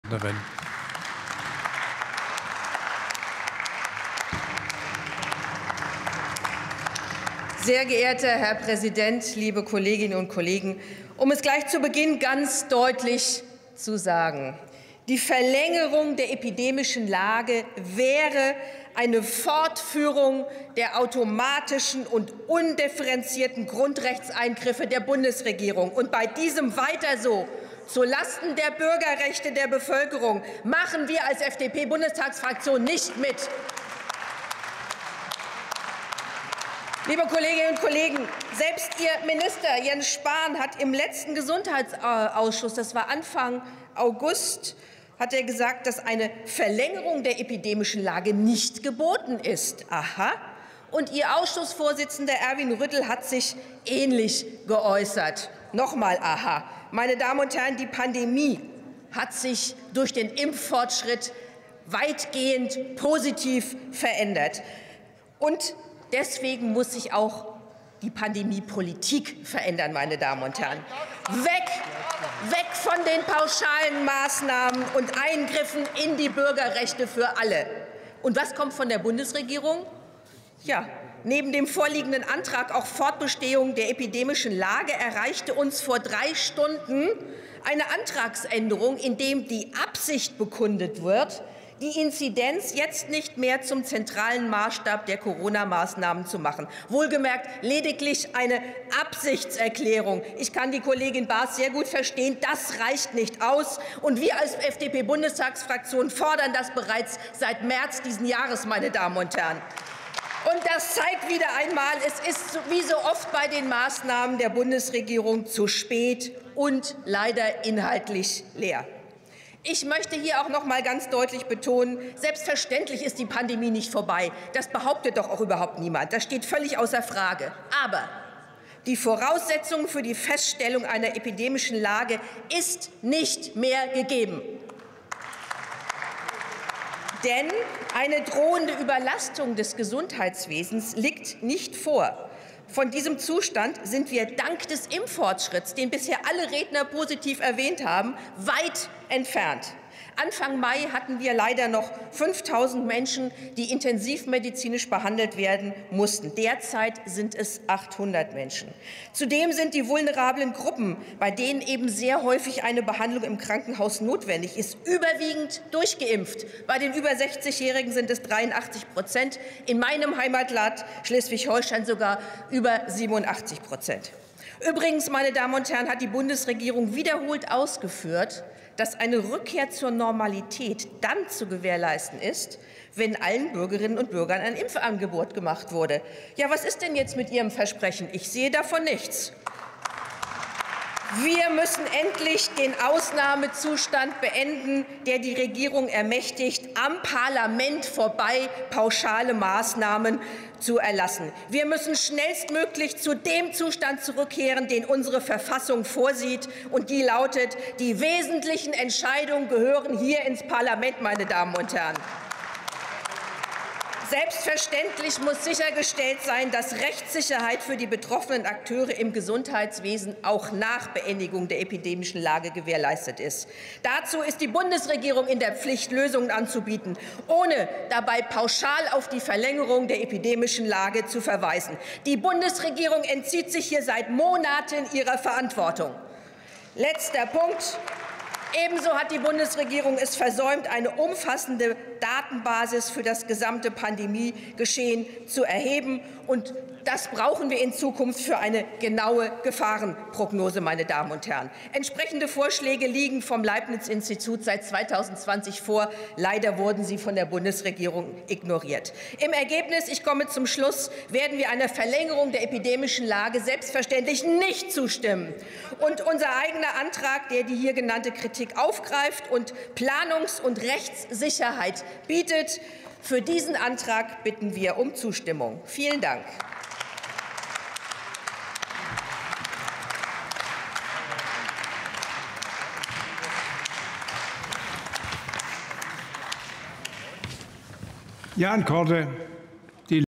EpidemischeLage beenden!
„Die Verlängerung der epidemischen Lage wäre eine Fortführung der automatischen und undifferenzierten Grundrechtseingriffe der Bundesregierung. Bei diesem „Weiter so“ zu Lasten der Bürgerrechte der Bevölkerung machen wir als FDP-Bundestagsfraktion nicht mit.

Die Pandemie hat sich durch den Impffortschritt weitgehend positiv verändert und deswegen muss sich auch die Pandemiepolitik verändern. Weg von den pauschalen Maßnahmen und Eingriffen in die Bürgerrechte für alle! Selbstverständlich ist die Pandemie nicht vorbei, aber die Voraussetzungen für die Festsetzung einer epidemischen Lage nationaler Tragweite ist nicht mehr gegeben. Denn eine drohende Überlastung des Gesundheitswesens liegt nicht vor. Von diesem Zustand sind wir dank des Impffortschritts weit entfernt. Anfang Mai gab es noch 5.000 Menschen, die intensivmedizinisch behandelt werden mussten. Aktuell sind es nur noch 800. Zudem sind die vulnerablen Gruppen überwiegend durchgeimpft.

Wir müssen endlich den Ausnahmezustand beenden, der die Bundesregierung ermächtig am Parlament vorbei pauschale Maßnahmen zu erlassen. Wir müssen schnellstmöglich zu dem Zustand zurückkehren, den unsere Verfassung vorsieht und der lautet: Die wesentlichen Entscheidungen gehören in das Parlament. Selbstverständlich muss sichergestellt sein, dass Rechtssicherheit für die betroffenen Akteure im Gesundheitswesen auch nach Beendigung der epidemischen Lage gewährleistet ist. Dazu ist die Bundesregierung in der Pflicht, Lösungen anzubieten – ohne dabei pauschal auf die Verlängerung der epidemischen Lage zu verweisen. Die Bundesregierung entzieht sich hier seit Monaten ihrer Verantwortung!“, so unsere gesundheitspolitische Sprecherin Christine Aschenberg-Dugnus zur epidemischenLage im Bundestag.